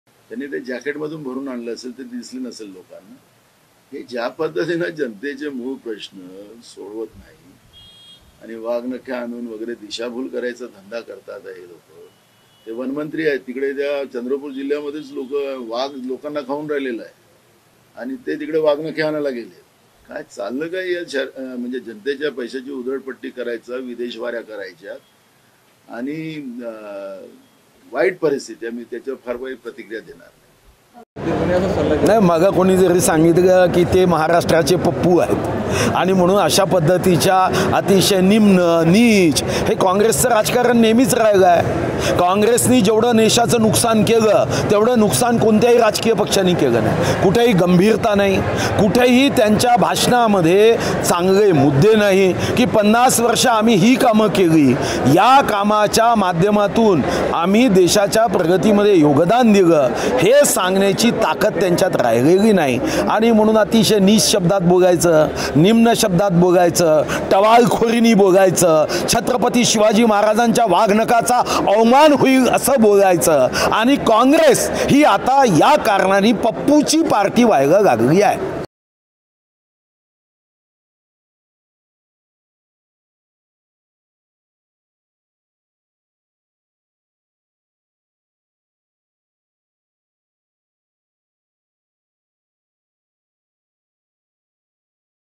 Sudhir Mungantiwar on Patole | वाघ नखांवरुन पटोलेंनी डिवचले, मुनगंटीवार भडकले | LetsUpp Marathi
वाघ नखांच्या मुद्यावरुन नाना पटोलेंनी घेरताच सुधीर मुनगंटीवार चांगलेच भडकले.